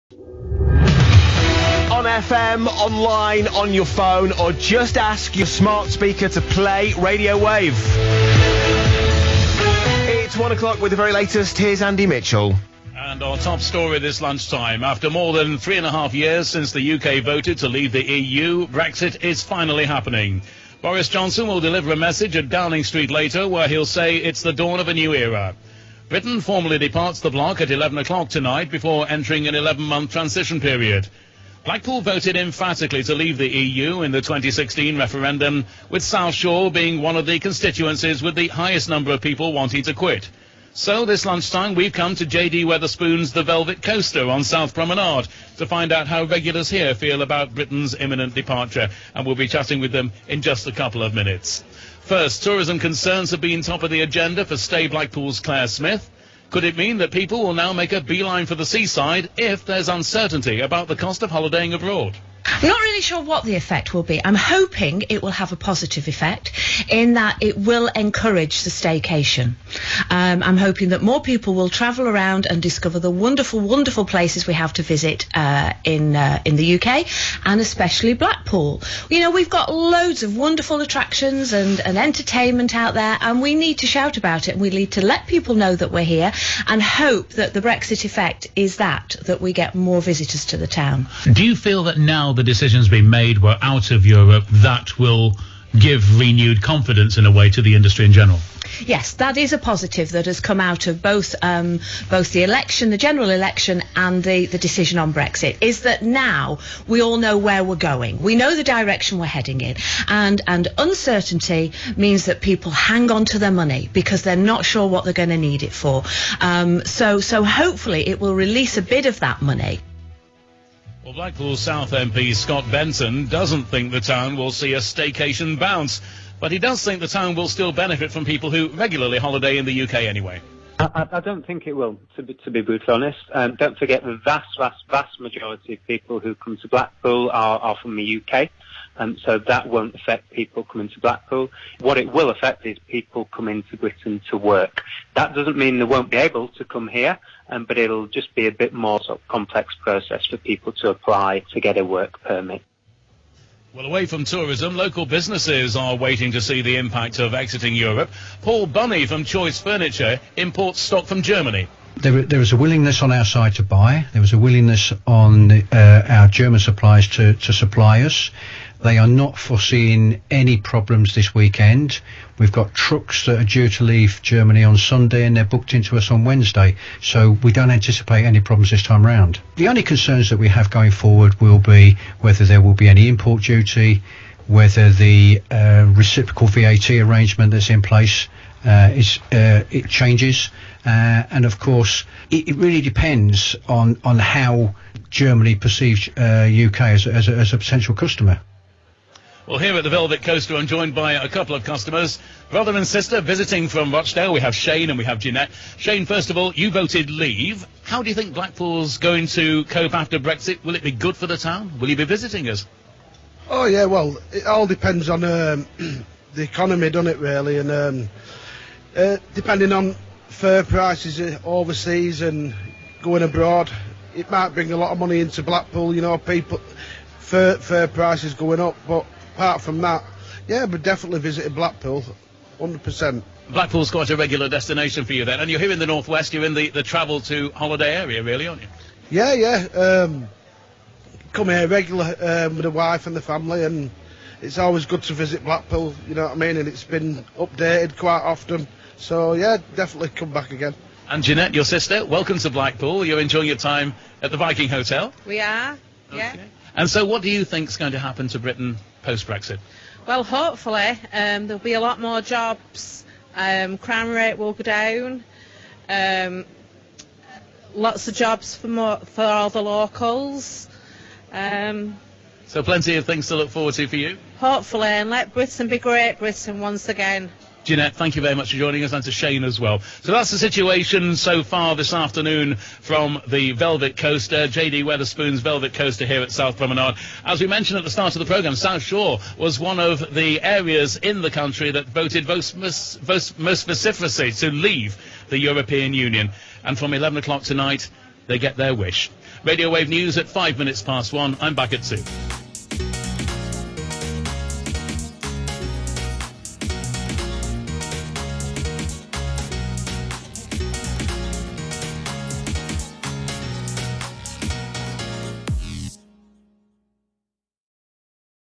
Brexit Eve Lunchtime News
NEWS:- On the eve of the UK leaving the EU, the Radio Wave Lunchtime News was live from the heart of the Brexit vote in Blackpool and the FyldeCoast. The News Team headed to the VelevetCoaster to find out what Brexit has in store for Blackpool.